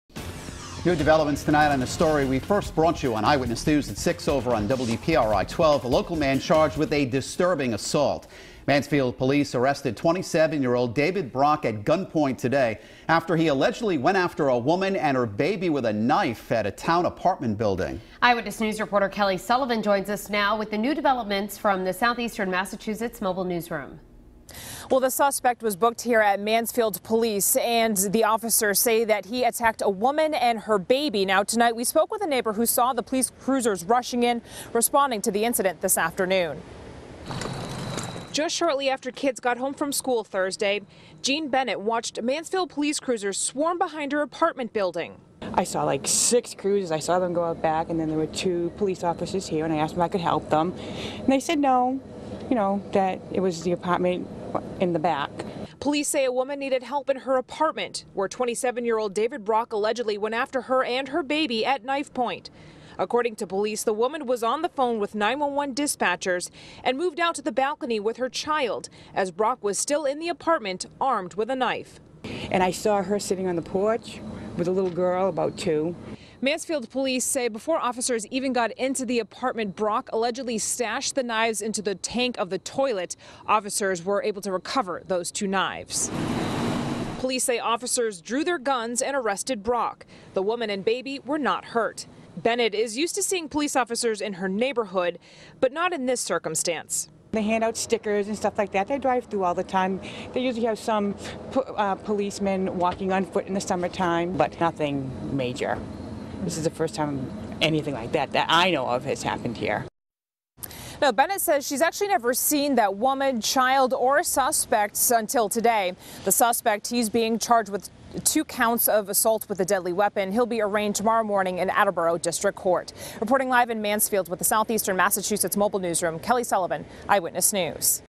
Police: Man arrested after attacking woman, child with knife
A man has been charged after police said he allegedly went after a woman and her baby with a knife.